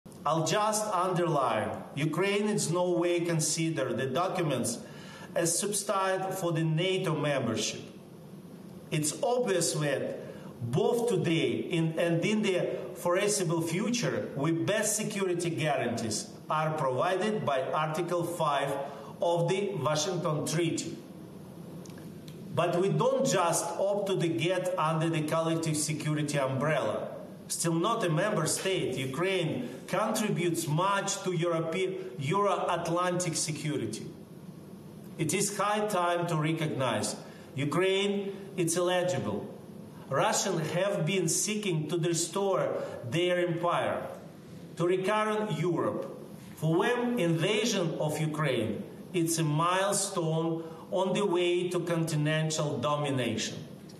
Ucraina: Andriy Yermak, "Kiev contribuisce a sicurezza atlantica, è il momento di riconoscerlo"
È quanto detto dal capogabinetto del presidente ucraino Zelensky, Andriy Yermak, durante l’audizione congiunta della commissione per gli Affari esteri e della sottocommissione per la Sicurezza e la difesa, riunite per discutere sul rafforzamento del sostegno dell'Ue all'Ucraina.